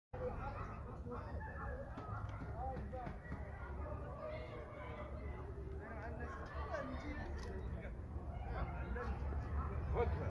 fat monkey can't resist the fresh food
In this video, you see how monkeys can't resist fresh food in a nice scene 🥰 Enjoy watching 🥰🥰